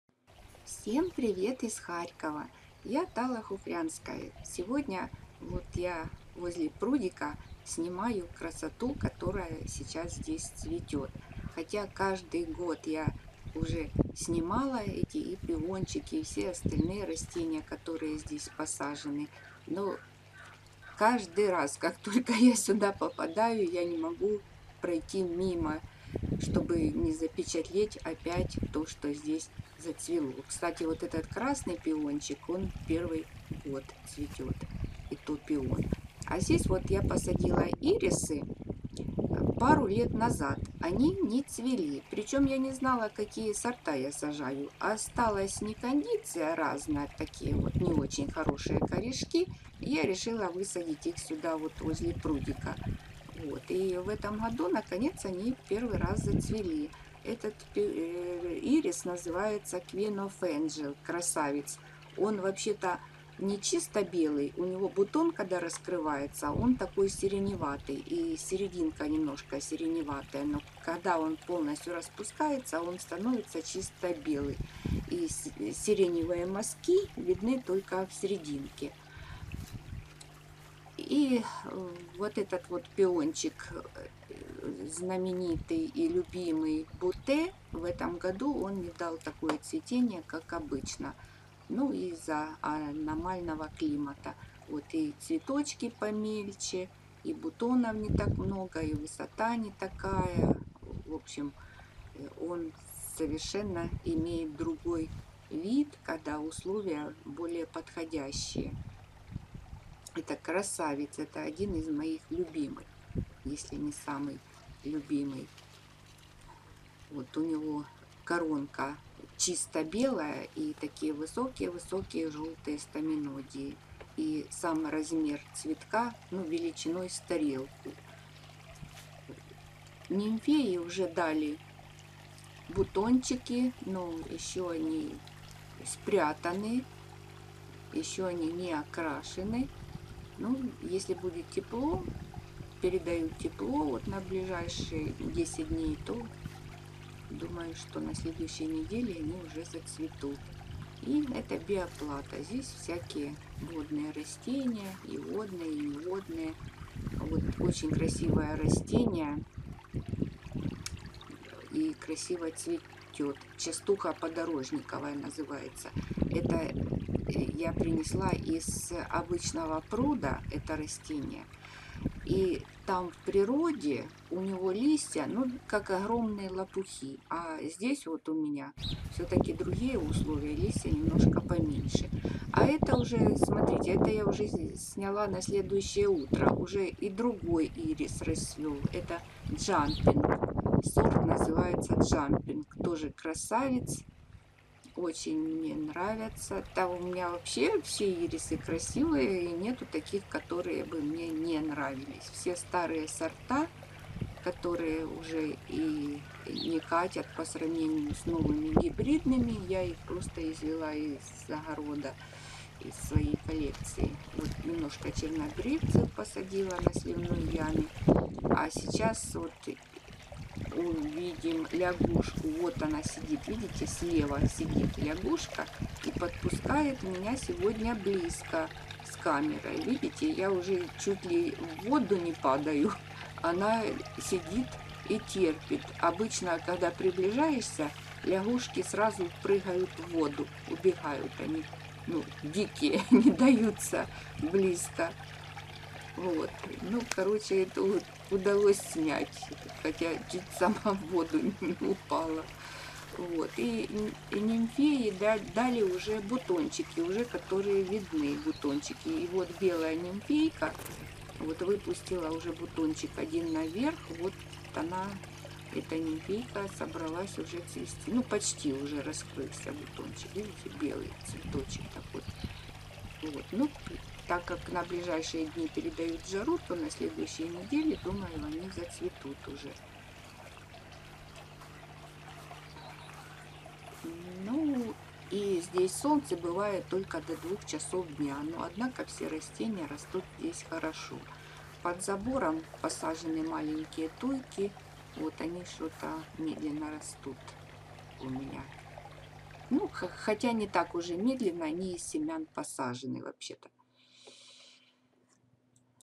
Пруд. Какие цветы расцвели возле пруда на сливной яме
Пруд. Какие цветы расцвели возле моего плёночного пруда на сливной яме.
Неравнодушные гости канала могут оказать помощь моим многочисленным животным:
$ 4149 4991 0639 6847 Хухрянская Н.В.